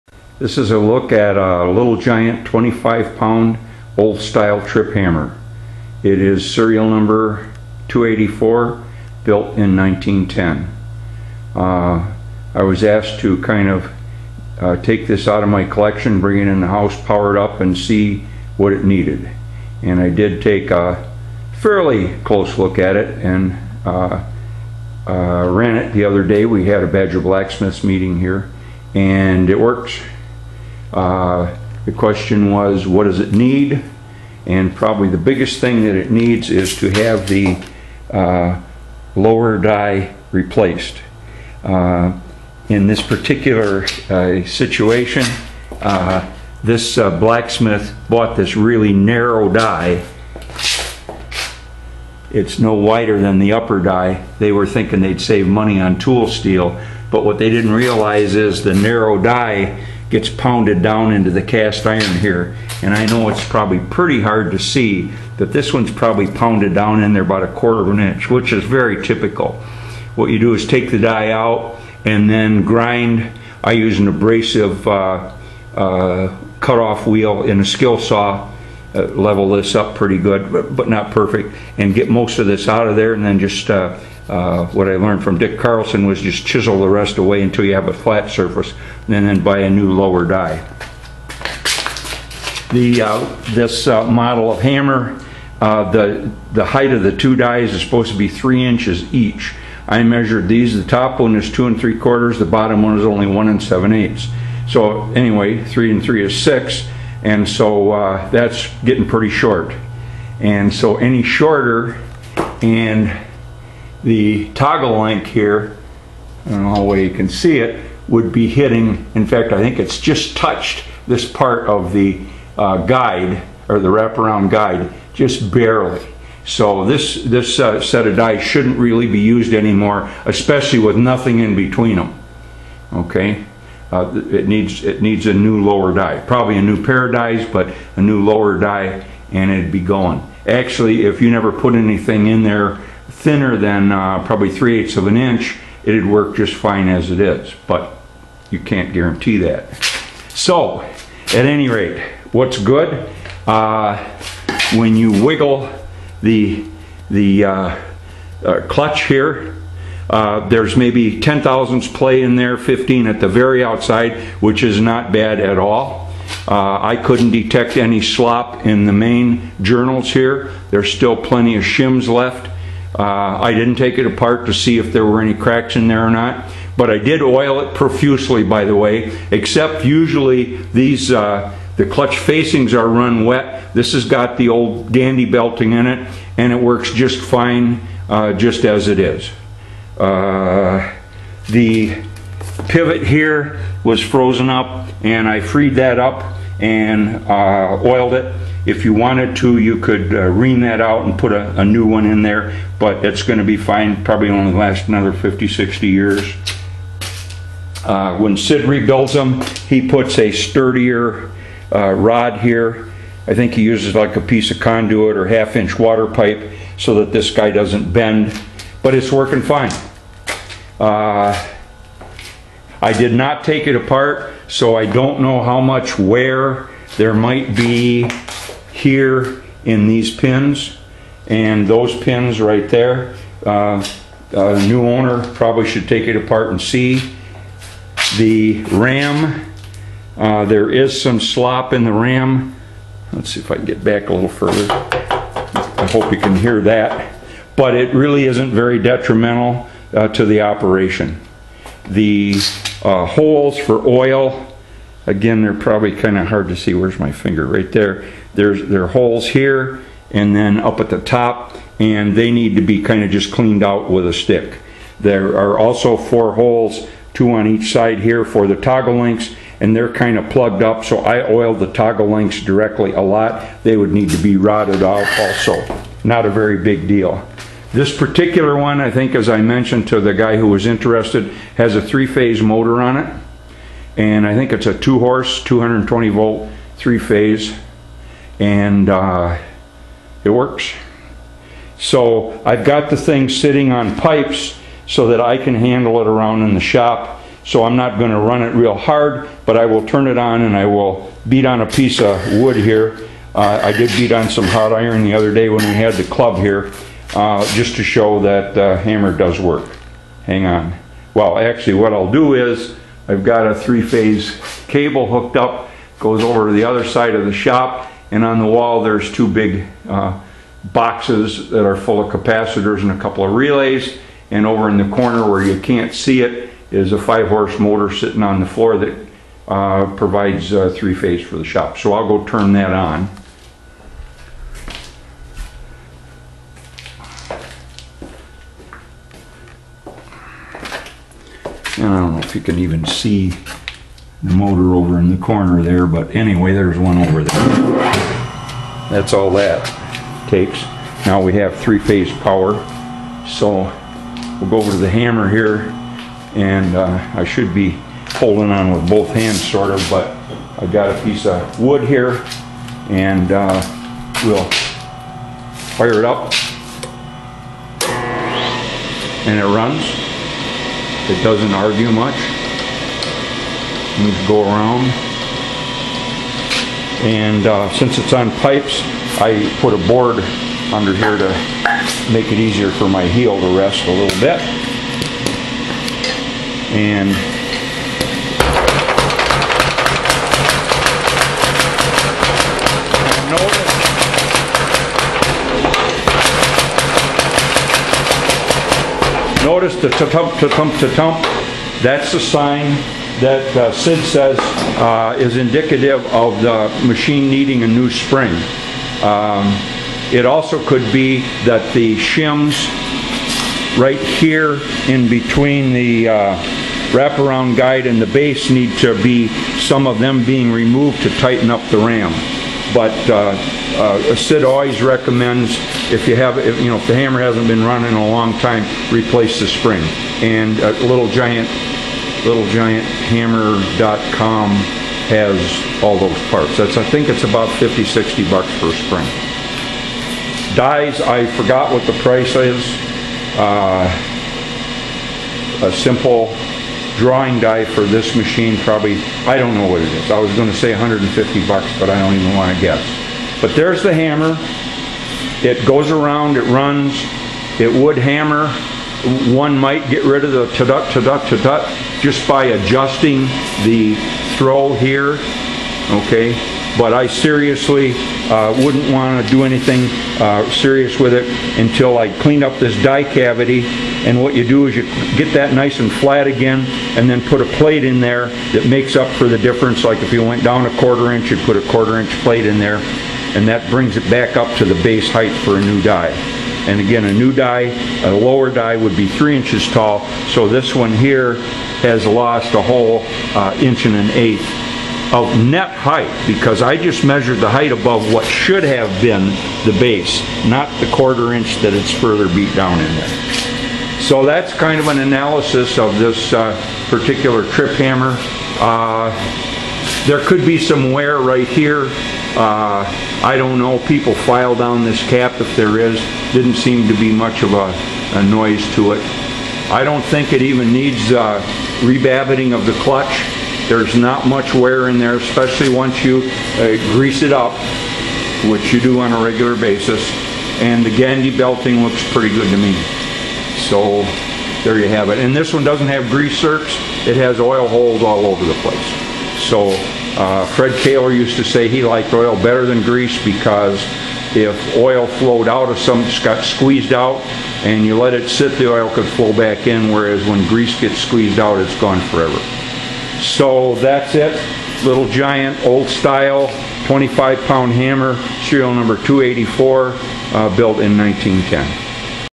25 Pound Little Giant Trip Hammer CheckOut
Power hammer evaluation.  Here I take one of the power hammers from my small collection and evaluate its condition.  It was made in 1910 and is what the call an "Old Style" Mayer Brothers Little Giant.  See more about trip hammers on our